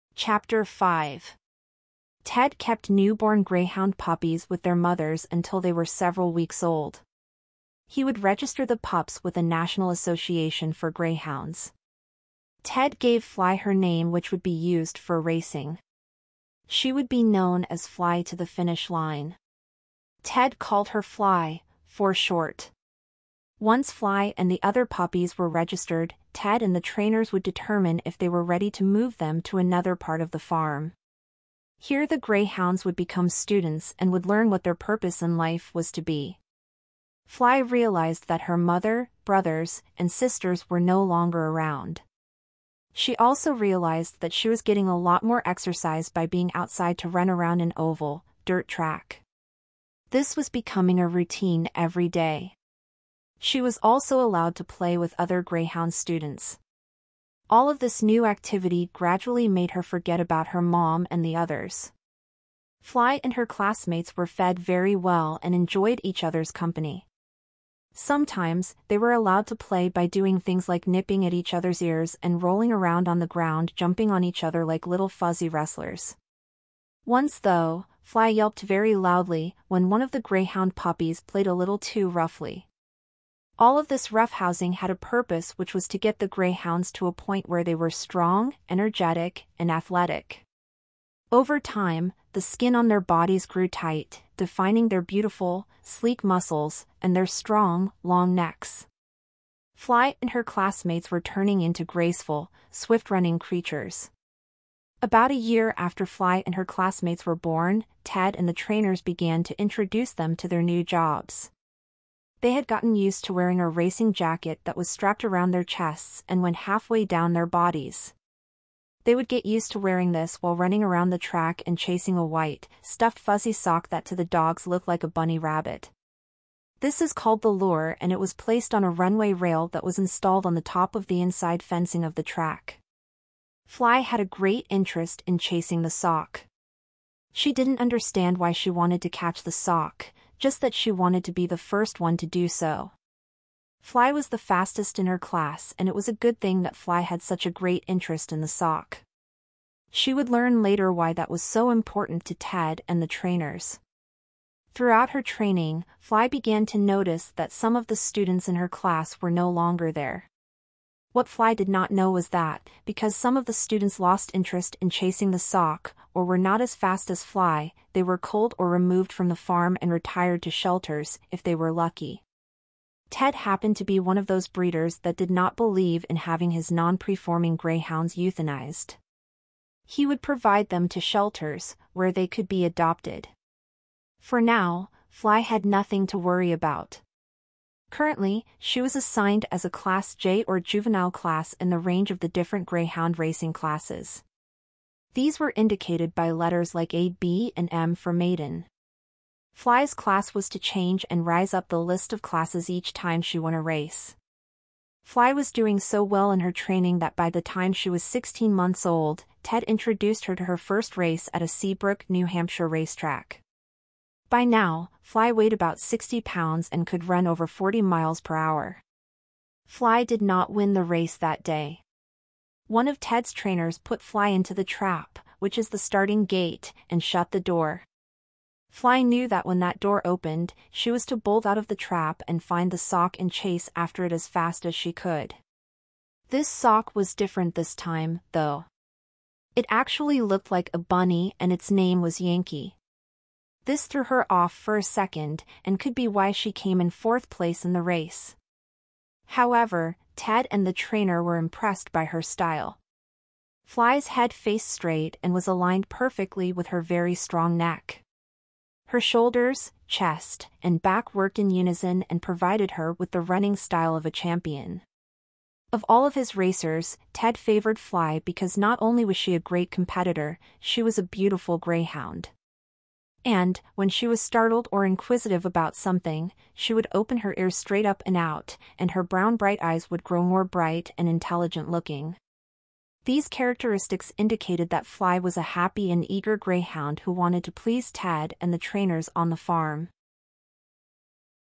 Fly, Greyhound Racer - Chapter 5 - Training
I hope you enjoy my story about my greyhound Fly. It is based on my personal experiences of having this wonderful dog in my life for several years. Fly did race at a New Hampshire dog racing track for almost 5 years. She was one of six puppies in her mother’s litter. We adopted Fly from a New Hampshire shelter. Over the years, Fly joined us on camping trips and other family outings. Fly and I walked outside, several times a day, and through all kinds of the New England weather. We walked through the woods in our neighborhood, and along the town sidewalks, getting to know everyone and the other dogs we met along the way. She was truly a joy in our lives.